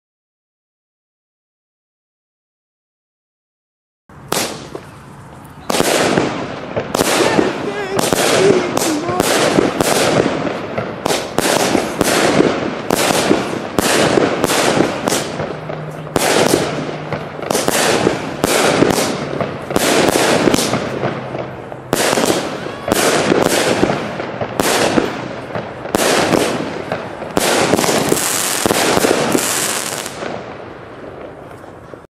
PGE Cake G Feuerwerksbatterie
Vorschiessen am 3.November 2012
Feuerwerksbatterie Cake G von Pyro Group Europe
25 Schuss NEM 256,4 g
Kategorie F2